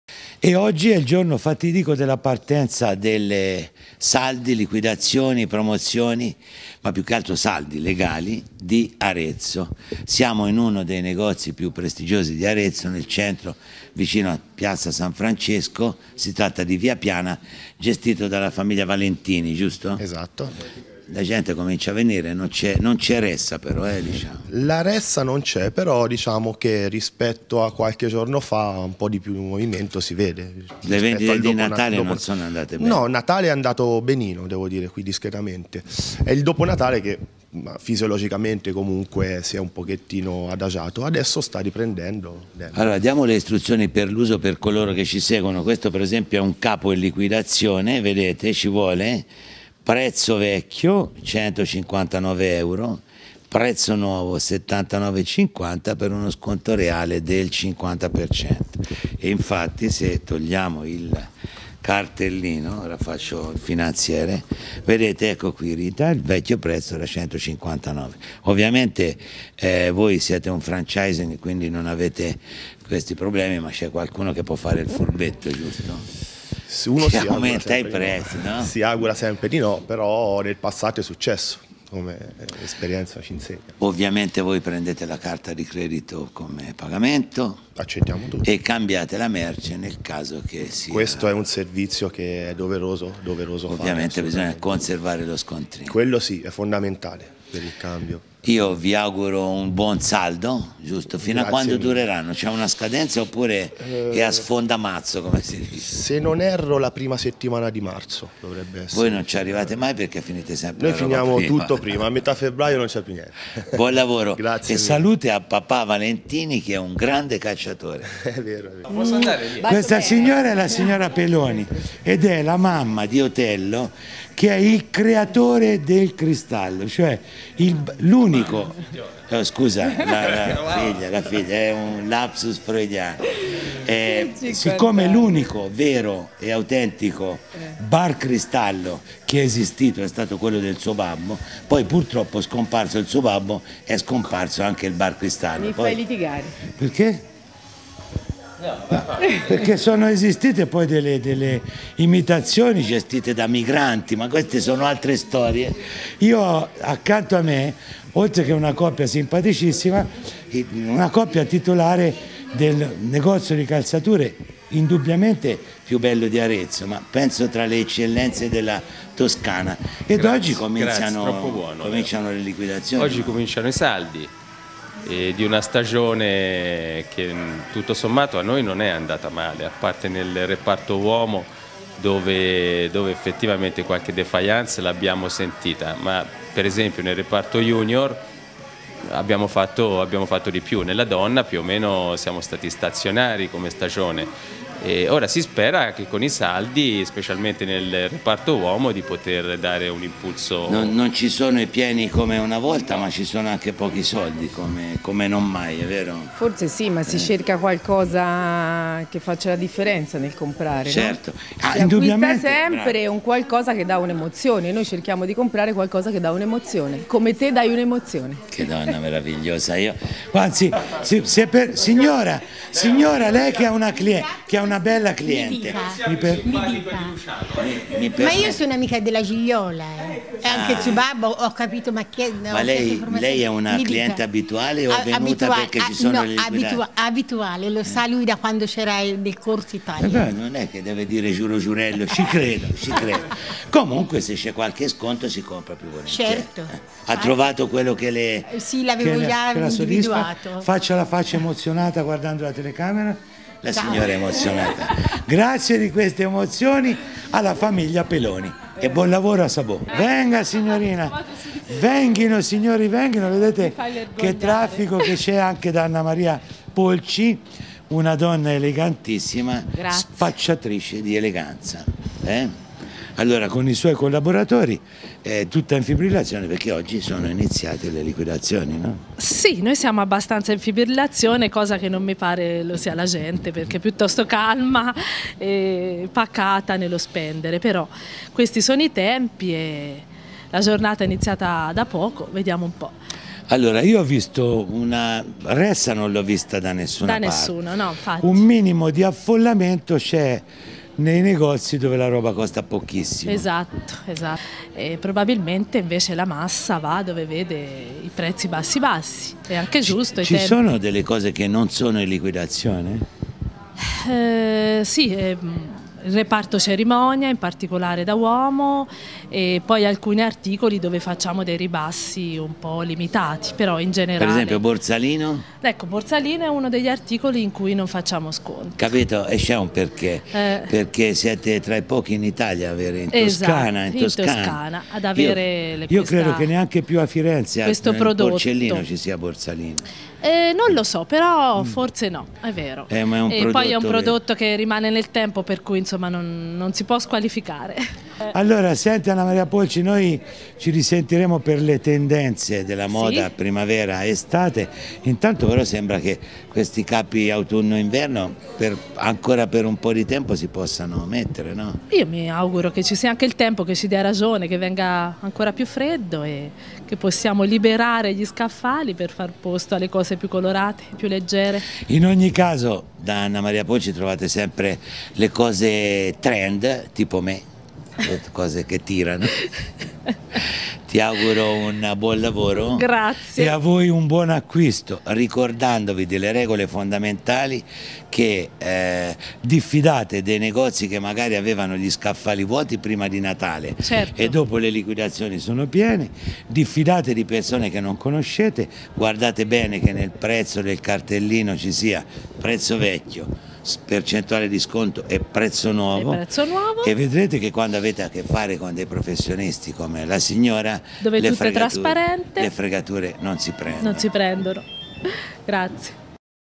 SALDI SALDI SALDI!
Per la Toscana oggi 5 Gennaio 2012 partono i saldi di fine stagione. Commercianti in attesa e clienti ancora un po' indecisi.
Come al solito attenzione a quello che si acquista e dove in questo periodo, perche' qualche commerciante piu' astuto si puo' sempre incontrare ma non e' il caso dei commercianti aretini che stamattina siamo andati a trovare per sapere le loro impressioni su questo inizio dei saldi.